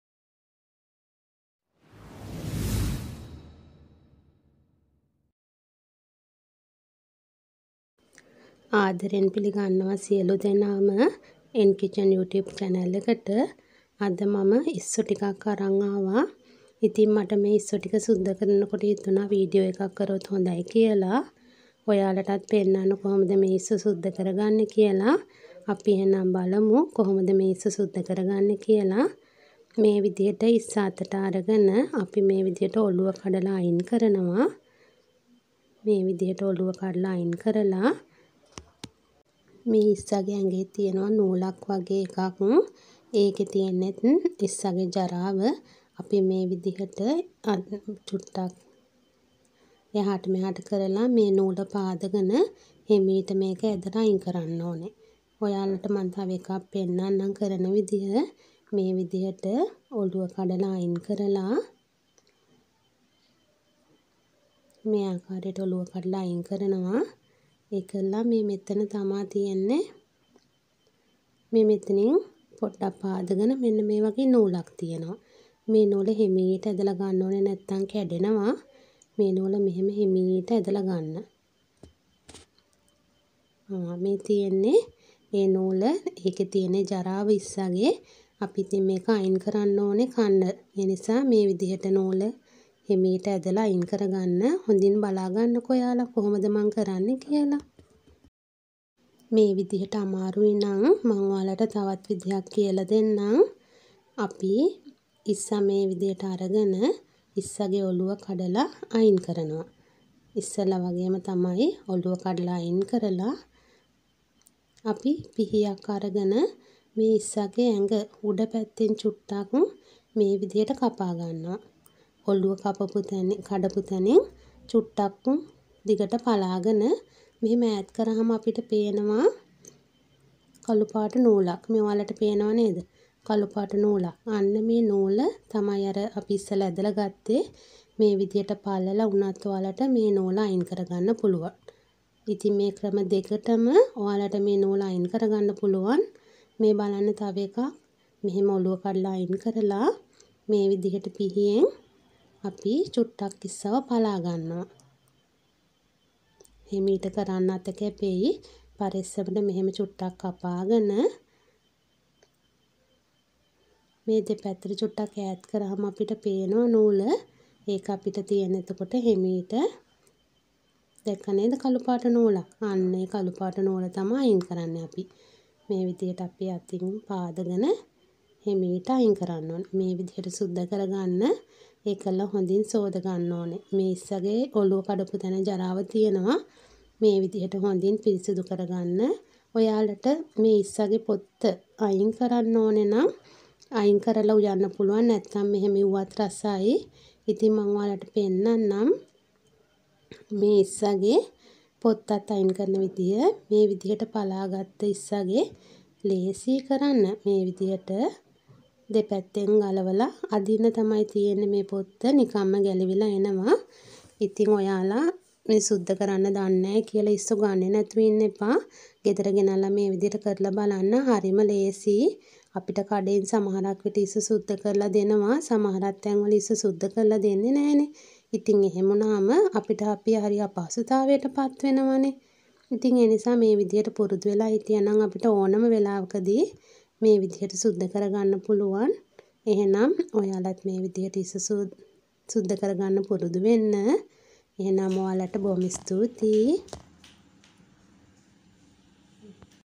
නිවැරදිව සුද්ද කරමු 😍prawn 😋
බොහෝ දෙනා නොදන්න ඉස්සන් සුද්ද කරන ලේසිම විදිහ